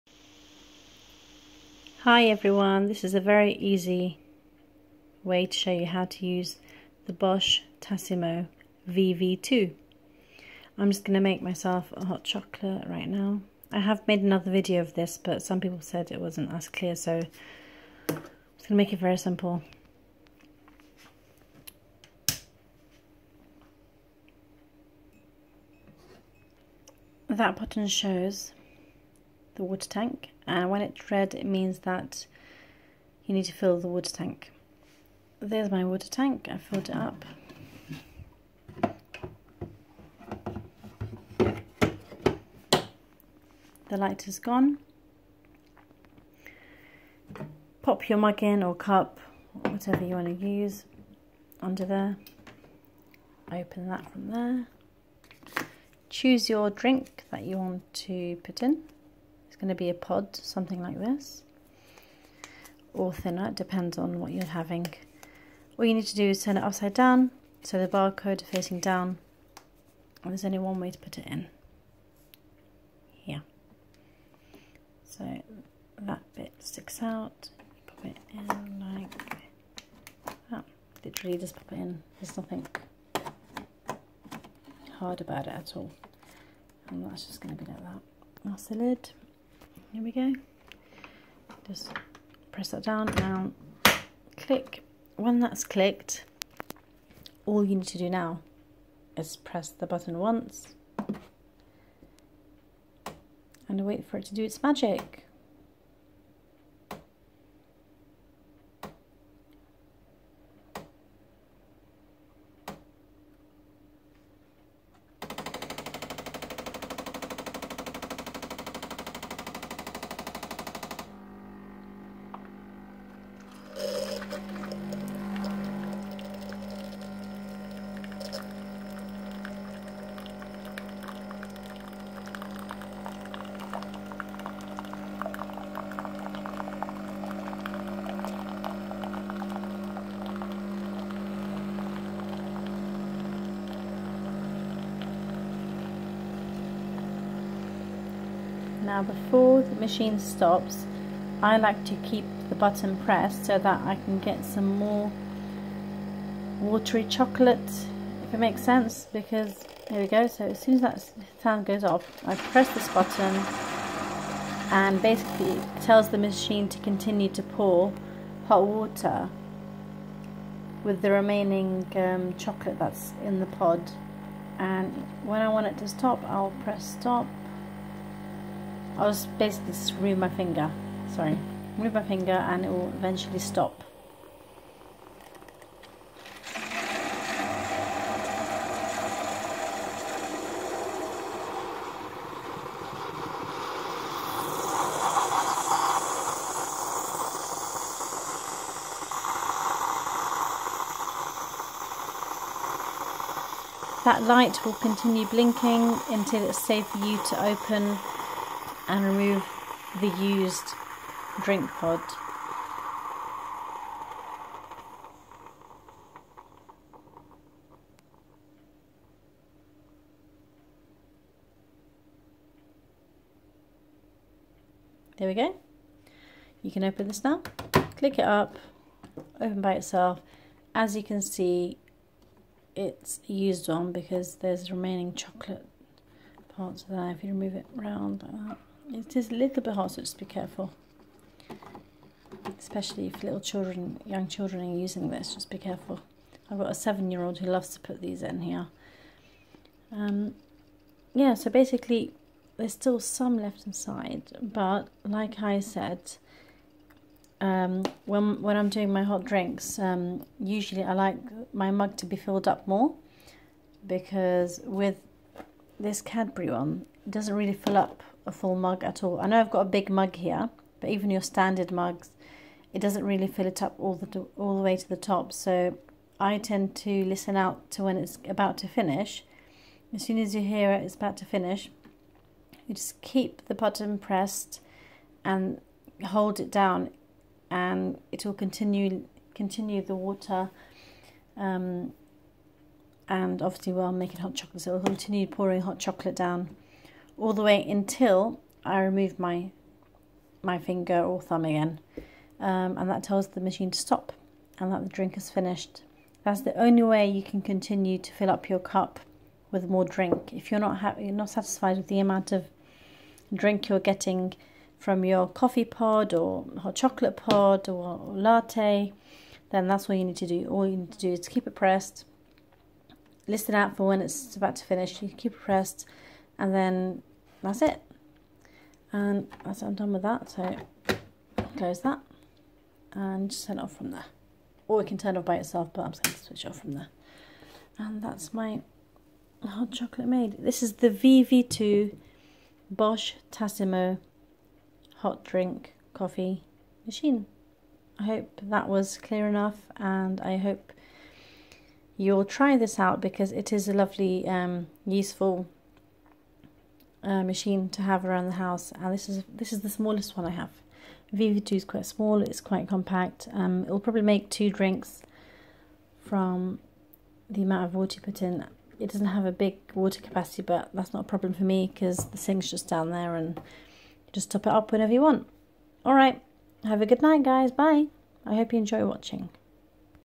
EASY TO FOLLOW INSTRUCTIONS ON HOW TO USE THE TASSIMO BOSCH VIVY 2 HOT DRINK MACHINE..
Simple instructions on how to use the bosch tassimo vivy 2 hot drink machine !